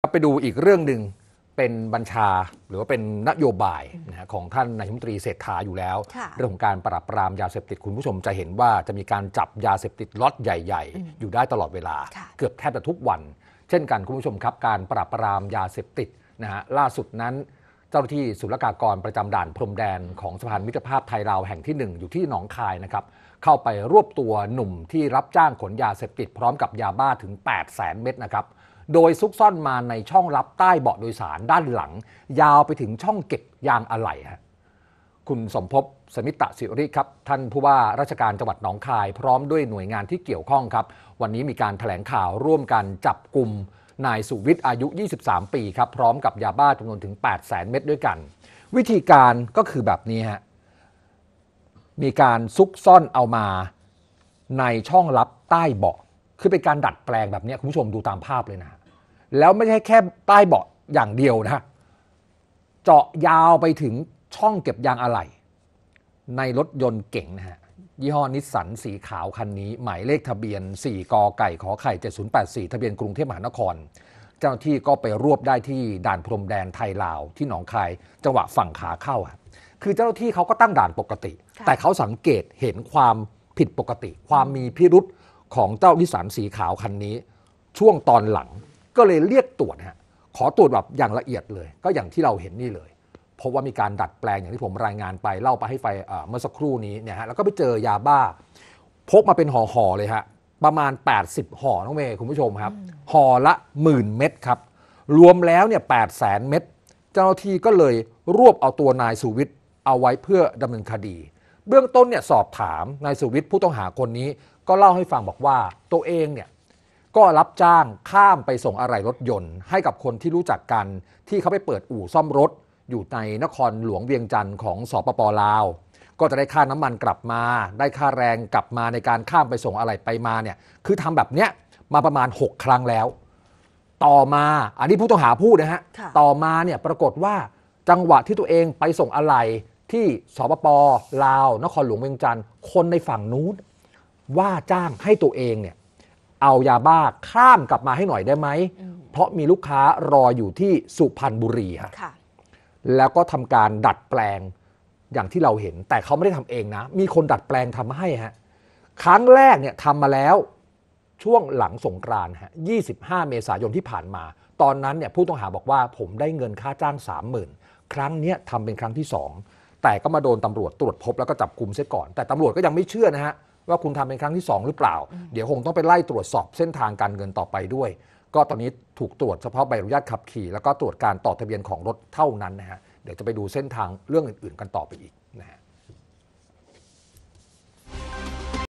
ยึดยาบ้า 8 แสนเม็ดคาด่านพรมแดนไทย-ลาว ข่าวดึก วันที่ 10 พฤษภาคม 2567
การเร่งปราบปรามยาเสพติด  ตามข้อสั่งการของนายกฯ ล่าสุดเจ้าหน้าที่ศุลกากร ประจำด่านพรมแดนสะพานมิตรภาพไทย-ลาว แห่งที่ 1 จังหวัดหนองคาย รวบหนุ่มรับจ้างขนยาเสพติด พร้อมยาบ้า 8 แสนเม็ด ที่ซุกซ่อนมาในช่องลับใต้เบาะโดยสารด้านหลังยาวไปถึงช่องเก็บยางอะไหล่